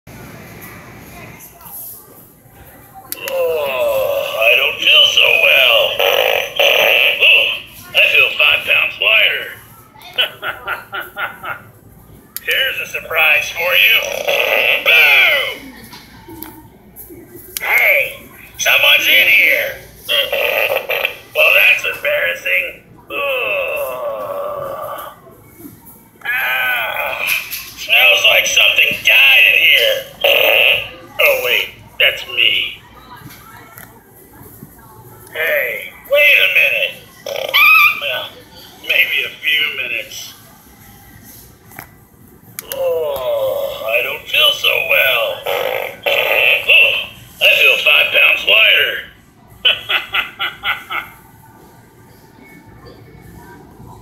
Haunted Toilet | Halloween 2016
Sold at Walmart for $14.97. Made by Gemmy Industries.

Recorded at:
Walmart